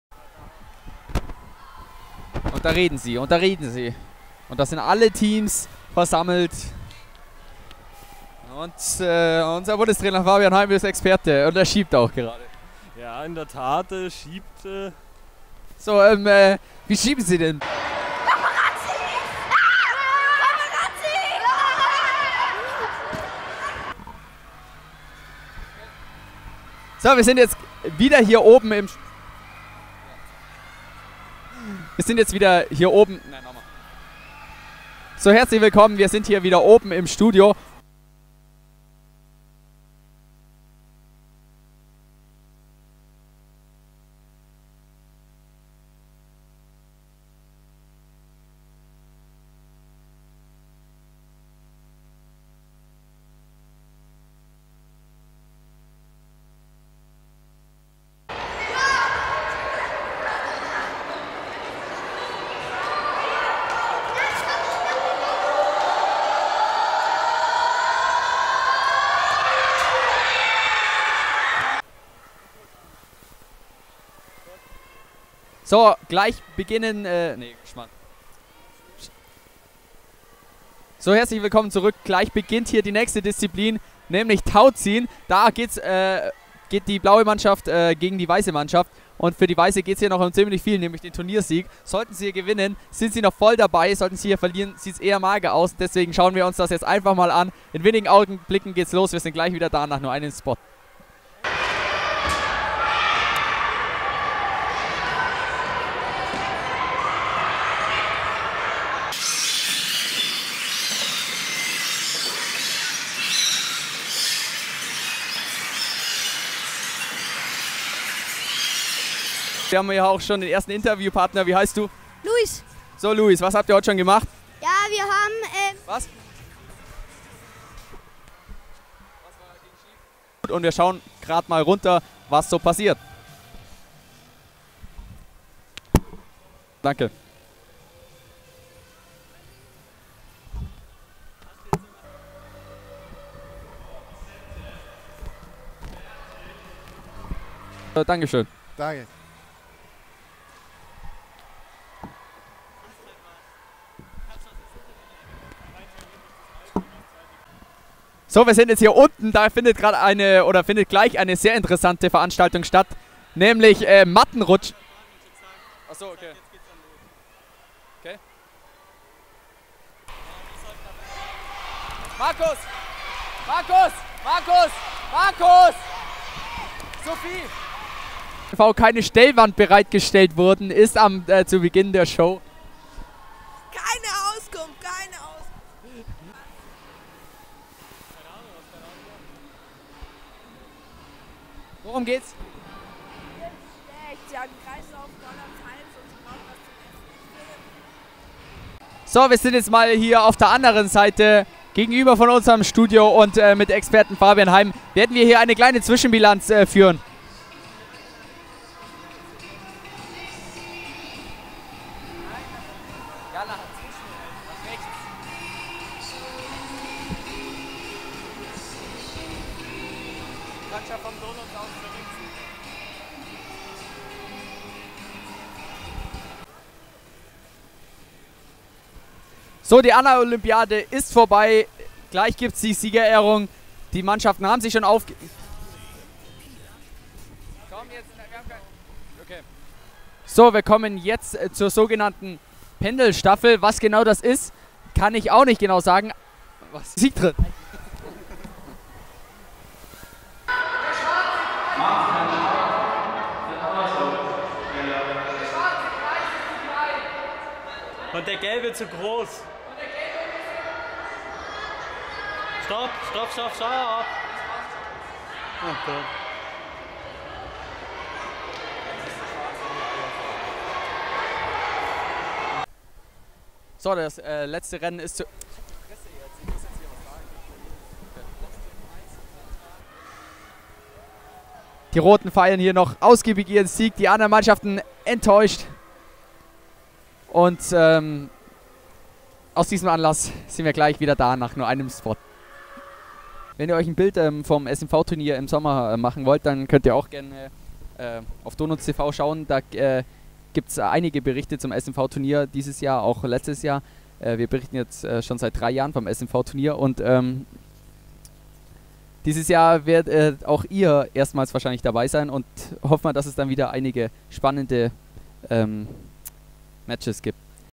Anna-Olympiade 2010 | Outtakes
Outtakes von der Anna-Olympiade am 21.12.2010 in der Sporthalle Augsburg.